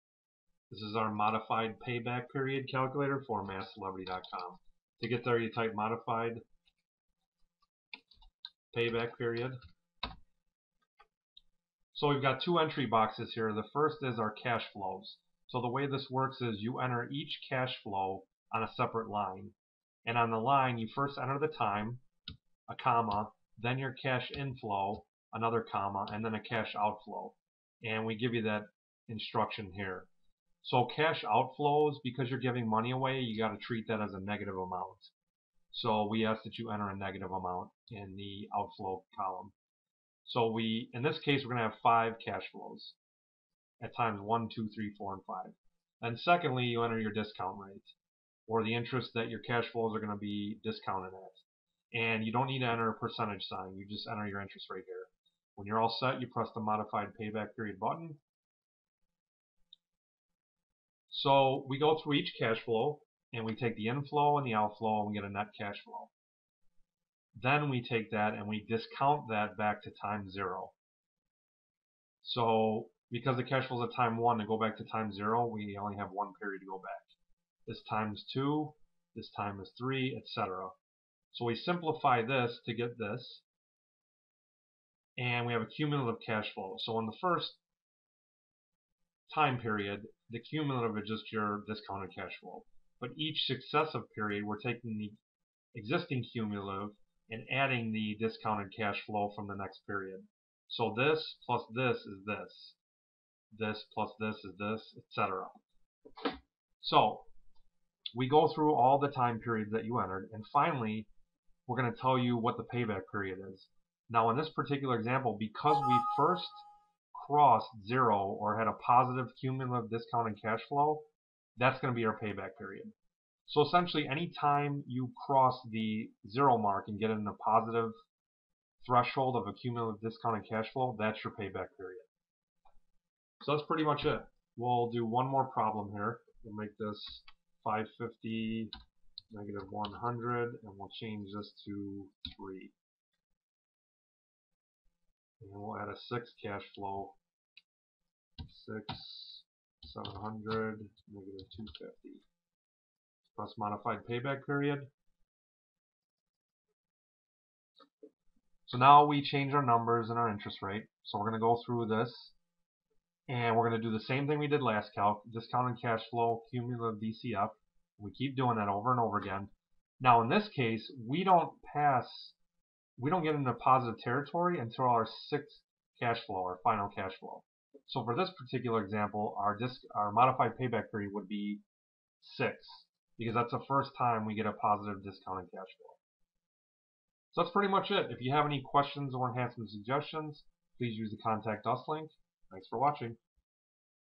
Modified Payback Period Calculator
Given a set of cash inflows, outflows as well as a discount rate, this calculates the modified payback period.

Calculator:
Get the tablet and products I use for math here